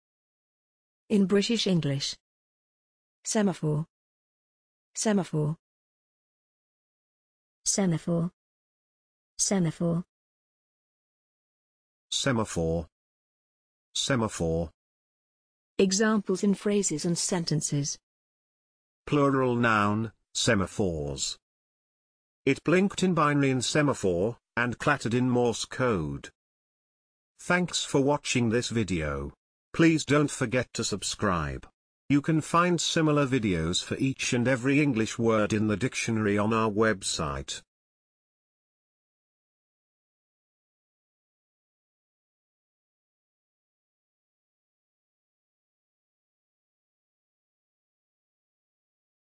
semaphore - pronunciation + Examples in sentences and phrases
This is the pronunciation of semaphore in British English + a few examples of its pronunciation in phrases and sentences.